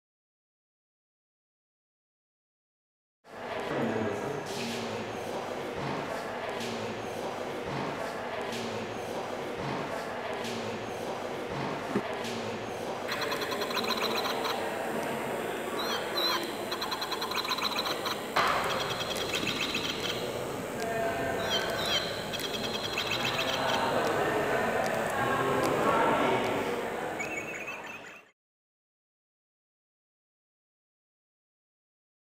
"Bird Language" by Xu Bing at The Fitzwilliam Museum
A small installation of four birdcages by contemporary artist Xu Bing. The birdcages are formed by metal words - questions and answers about the artist’s work. Make a noise around the artworks and the toy birds inside will revolve and emit sound in response. Xu Bing was the former President of the Central Academy of Fine Arts in Beijing, and has previously exhibited works at The British Museum, Victoria and Albert Museum and the Ashmolean Museum.

Wed 29 April 2015 to Sun 31 May 2015
Arts of the Far East (28)
Free